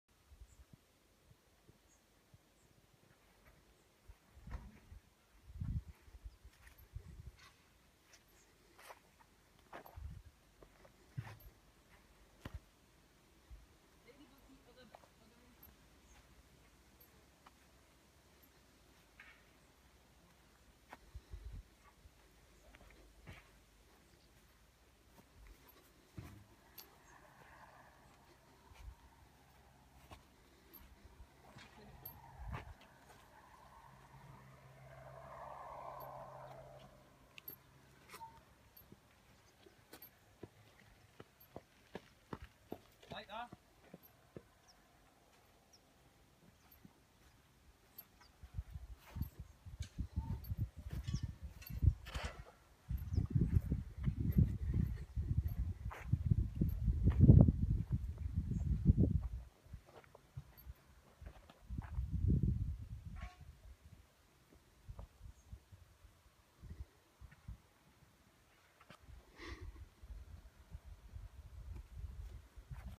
TrimTrail installation by first4playgrounds
first4playgrounds have recently installed a playground adventure trim trail at the Steiner Academy in Hereford.